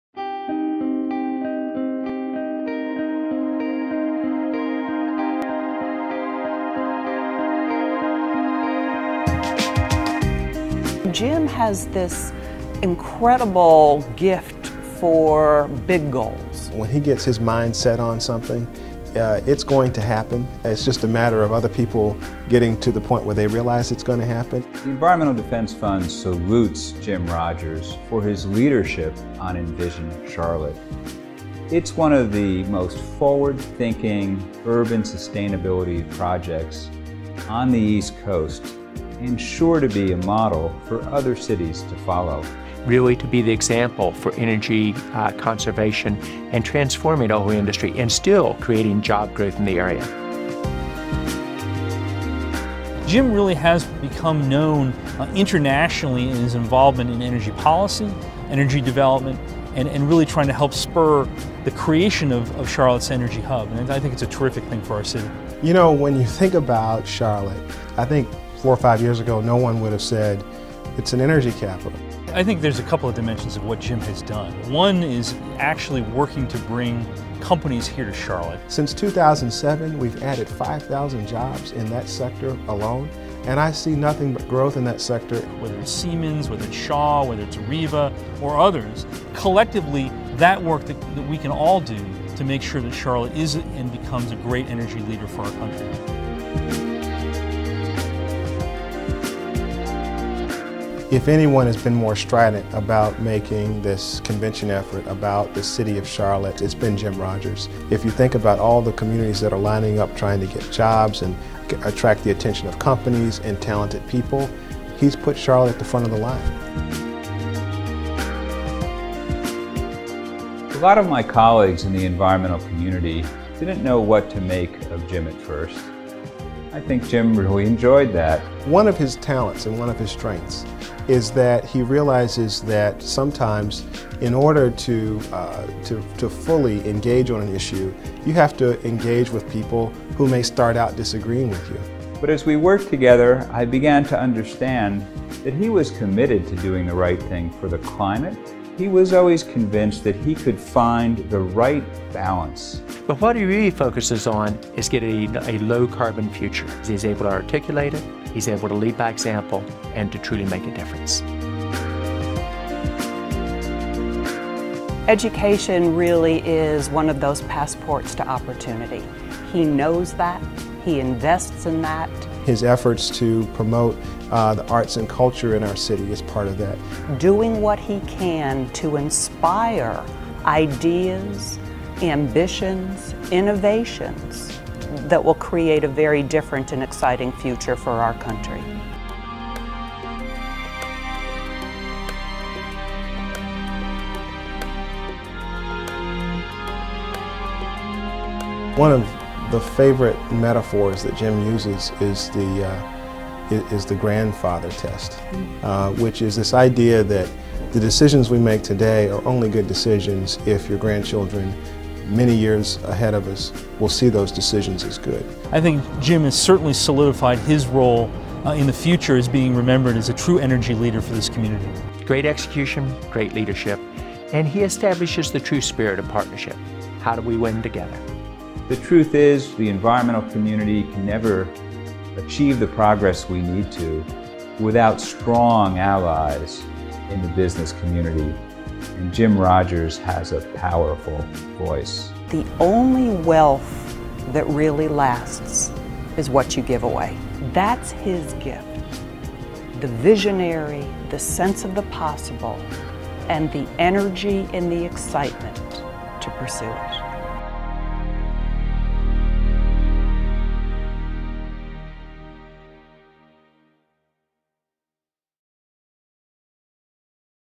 2012 Vision Award: Jim Rogers
The award recognizes a visionary and champion who has served as a major catalyst for Center City's emergence as a thriving urban core. Mr. Rogers one of our city's most prominent and inspiring city builders and executives who was instrumental in making Charlotte the successful place it is today.